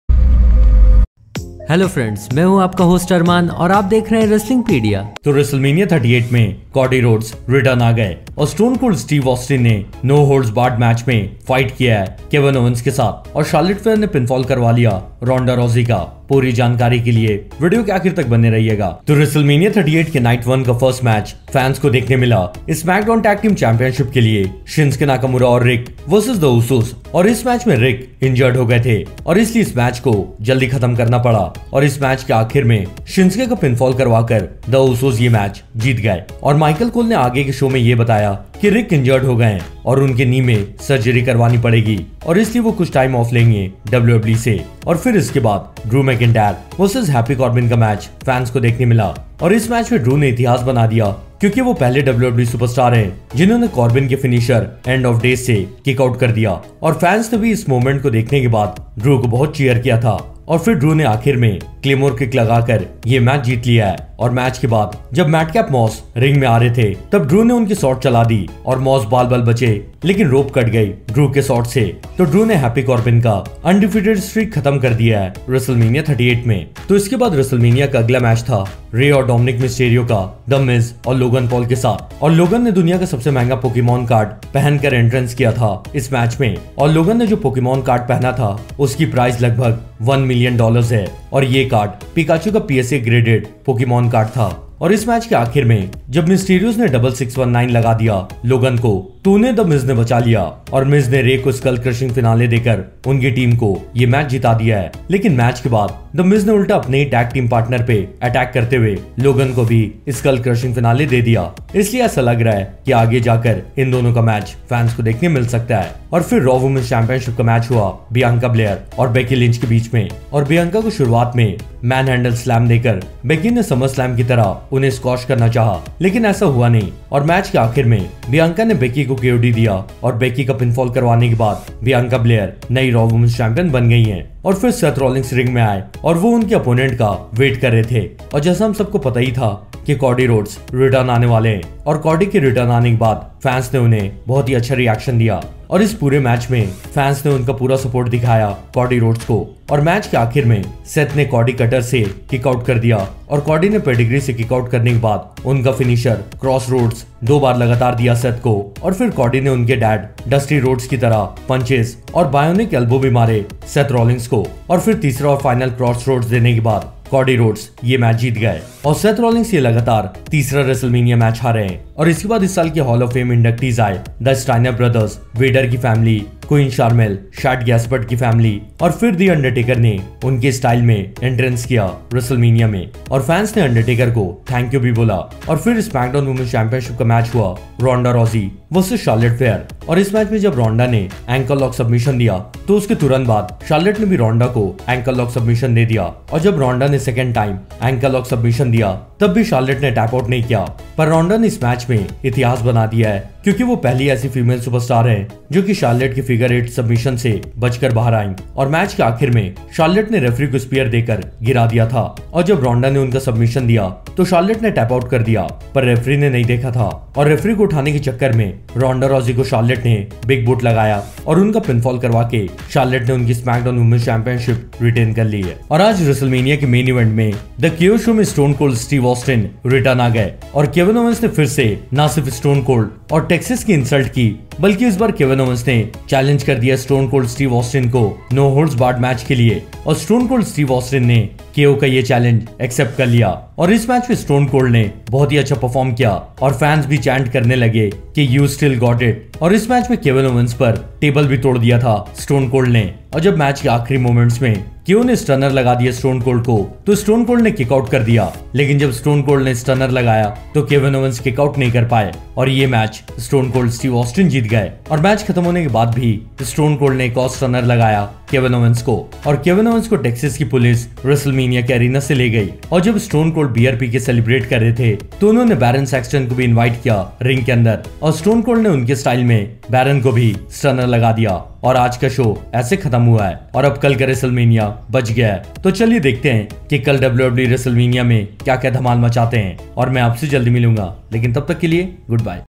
WWE Wrestlemania 38 New Champion Cody Return & Stone Cold In-Ring Return 2022
Watch the video to know about WRESTLEMANIA 38 NIGHT 1.